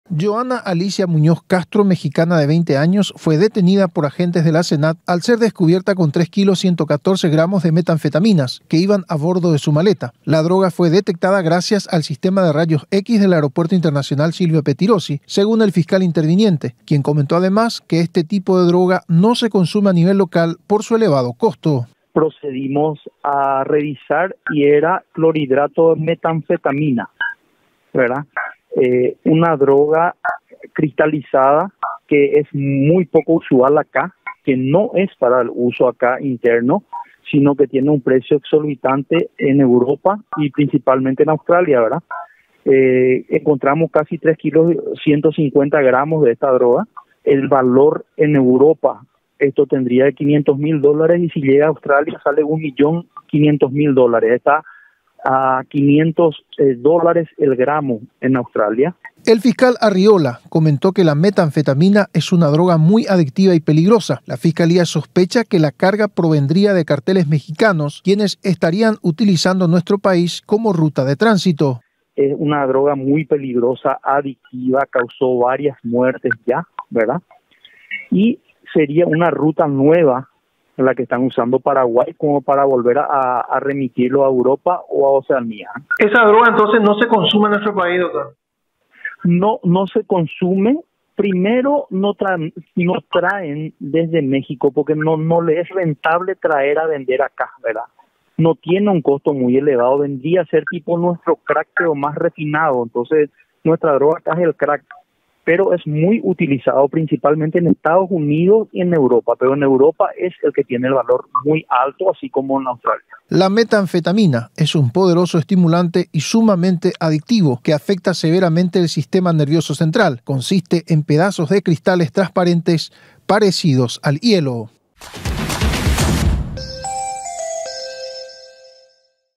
Detienen a mexicana con metanfetamina
Una ciudadana mexicana fue detenida en el aeropuerto internacional Silvio Pettirossi de nuestro país con más de tres kilos de metanfetaminas. Según los investigadores este tipo de drogas no se consume a nivel local  por lo que existen sospechas que narcotraficantes mexicanos están utilizando a Paraguay como país de tránsito de este tipo de droga según el fiscal Andrés Arriola.